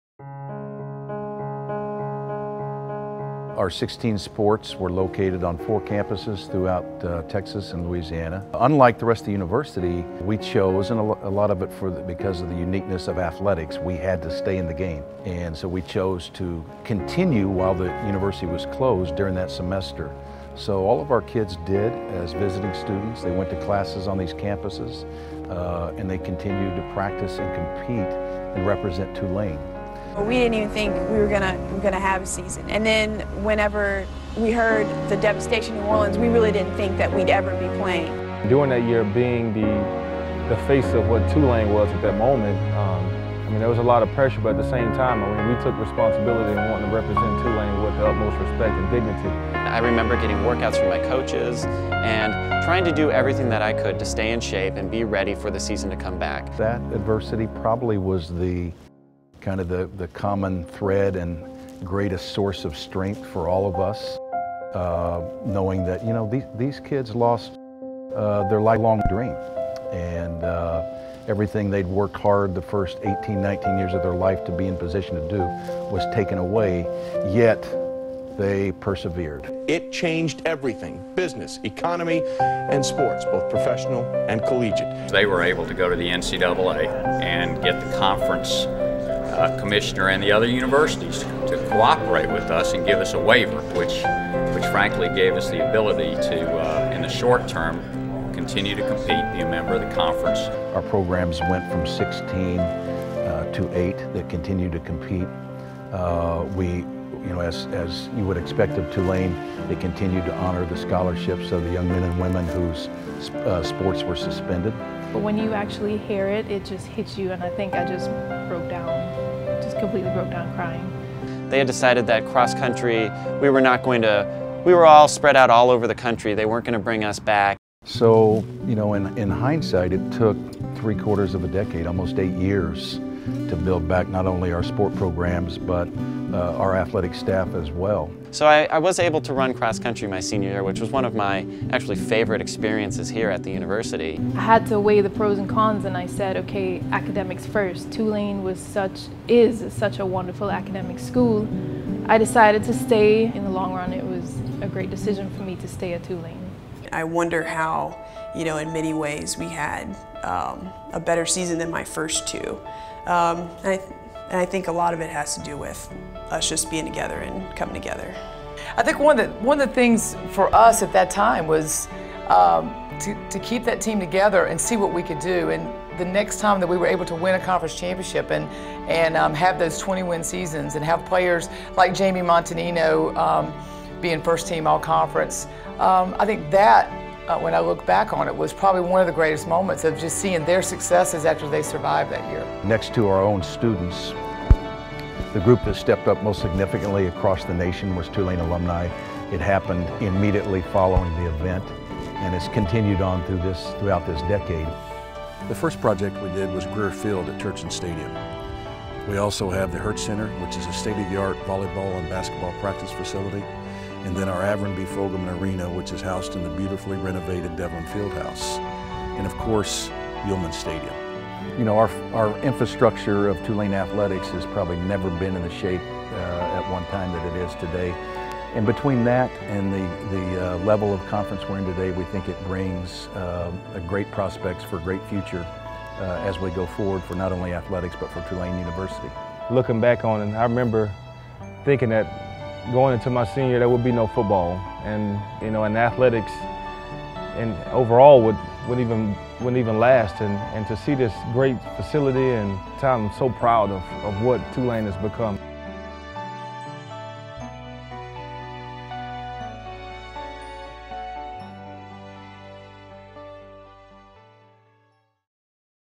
Athletics Carries the Torch
A look back at the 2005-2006 athletics season, when Hurricane Katrina’s aftermath caused a massive evacuation of New Orleans. Then-president Scott Cowen asked the Green Wave athletics teams “to carry the torch, be the face and represent the name” of the university. Tulane’s NCAA athletics programs were hosted by four different universities in the Fall 2005 semester and continued to compete though they had no home. Later that semester, the Renewal Plan, enacted in order to save Tulane financially, cut the sports programs down from 16 to 8, which was devastating to student-athletes all over again.

Today, Tulane is back up to 16 competing programs, and has new facilities for baseball, volleyball and basketball practice facilities, newly renovated basketball arena, and an on-campus football stadium. Featuring Tulane athletic director Rick Dickson, women’s basketball coach Lisa Stockton, and student-athletes Jami Montagnino, Lester Ricard, Tim Cullen, Linda McEachrane.

Produced by Mary Mouton, videography and editing by Jake Springfield.